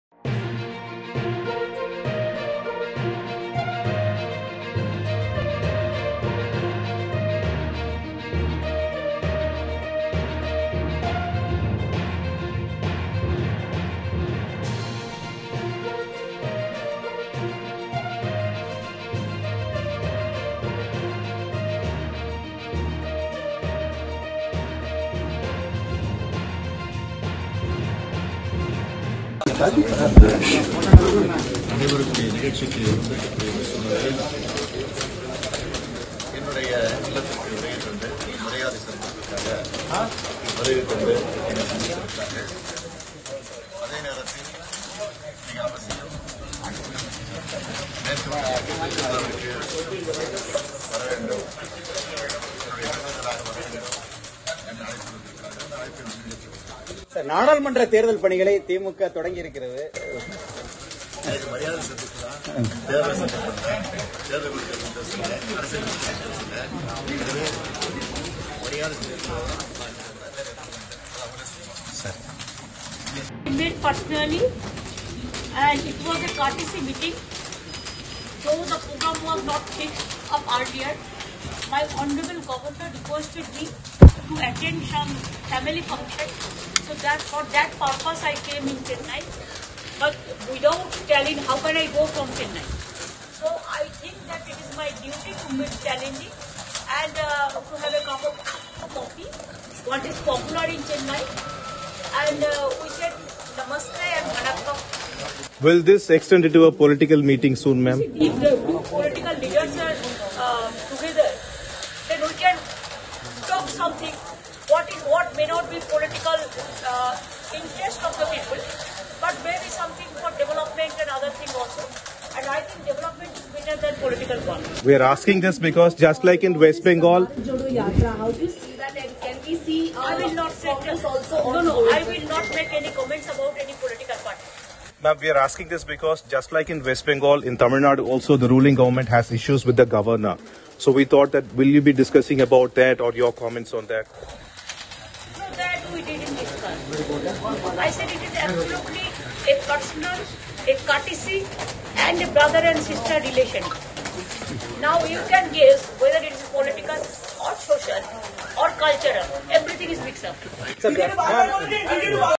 West Bengal CM Mamata Banerjee visits MK Stalin | Chennai
West Bengal CM Mamata Banerjee visited her Tamil Nadu counterpart MK Stalin at the latter’s residence in the city.

The two leaders denied discussing politics during the meeting.

Video by Manivasagan N